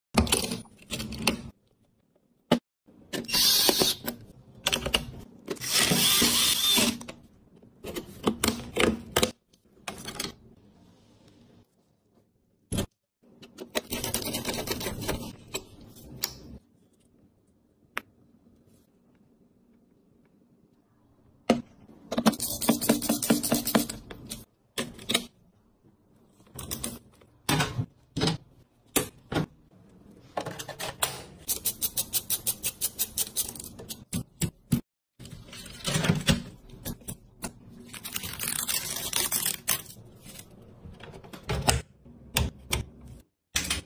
Intel i7-13790F👌 with RTX4070🔥 pcbuild ASMR
Configuration list

Radiator: Kyushu Fengshen Ice Cube AK620 Digital

Memory: Kingston Beast 16G/3200C18*2, total 32G

Hard drive: Samsung 990Pro IT M.2 NVMe PCI€4.0

Motherboard: ASUS PRIME Z790-P D4

Graphics card: ASUS ATS RTX4070 012G GAMINGX Megalodon

Power supply: Great Wall GX850 rated 850W gold medal full module

Chassis: ASUS TUF GAMING GT502

Case Fan: West Point Player 3D Fantasy (6 anyway)

Accessories: 1700/1800 correction buckle